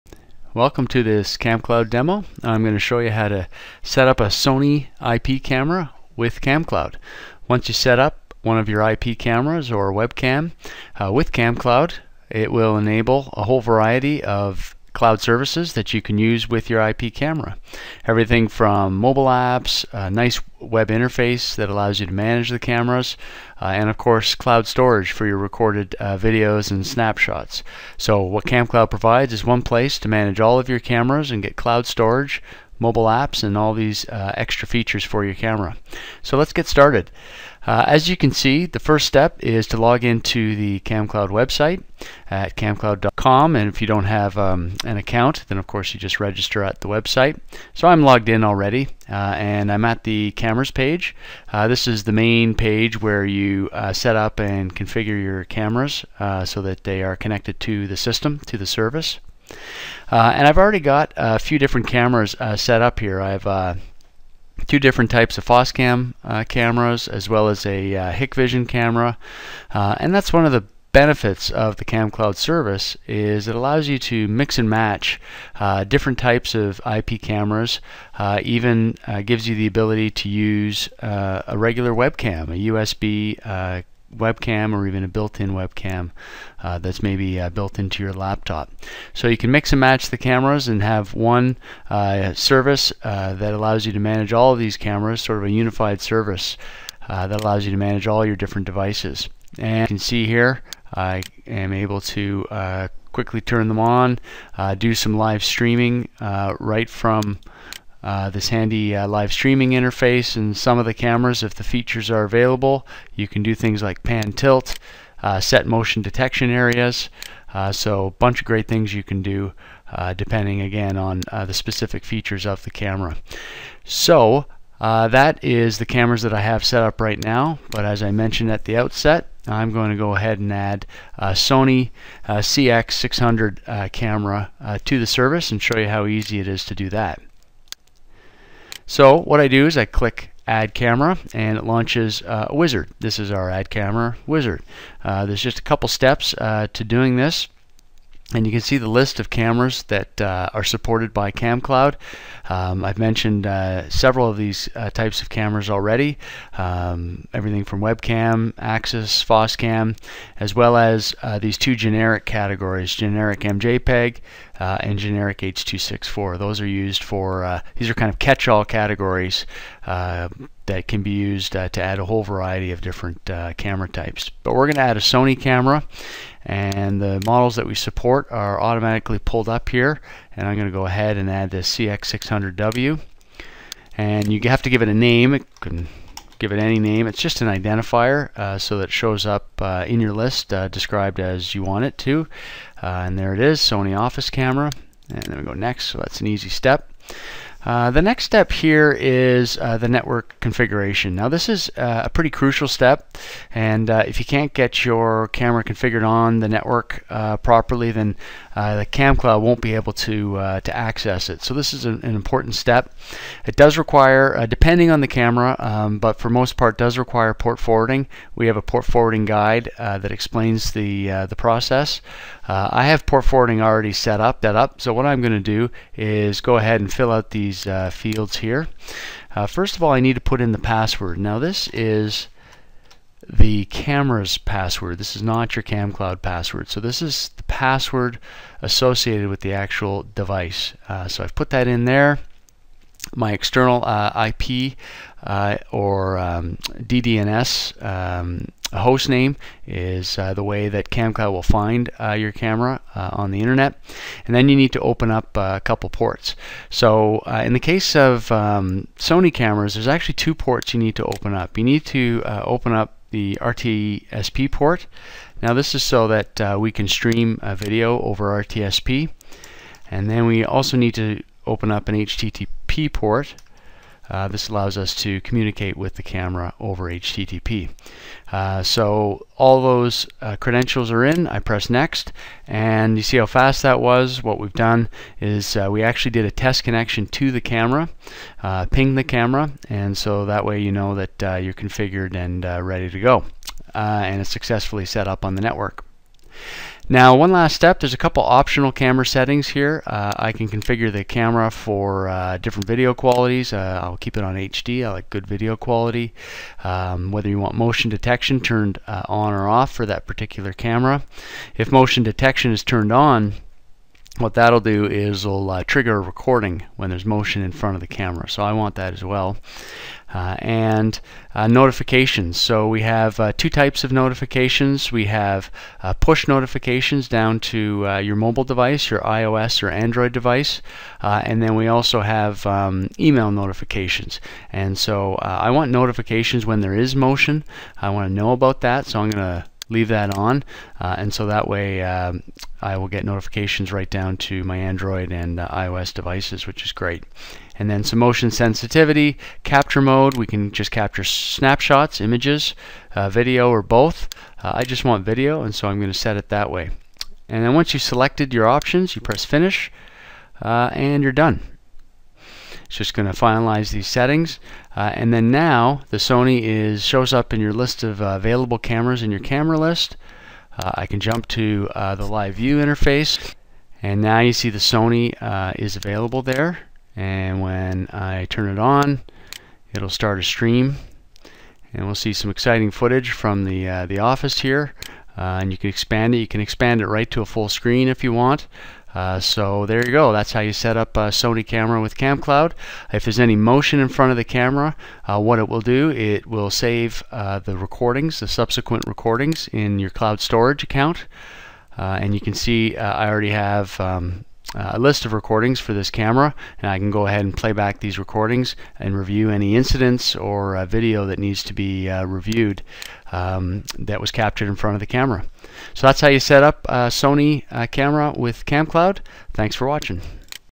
How to Setup a Sony IP Camera with Camcloud
This demo explains how to setup a Sony SNC-CX600W IP camera with Camcloud's cloud video monitoring service. Once you connect the Sony IP camera to Camcloud, you can take advantage of a range of video surveillance features including cloud storage, mobile apps and live video viewing.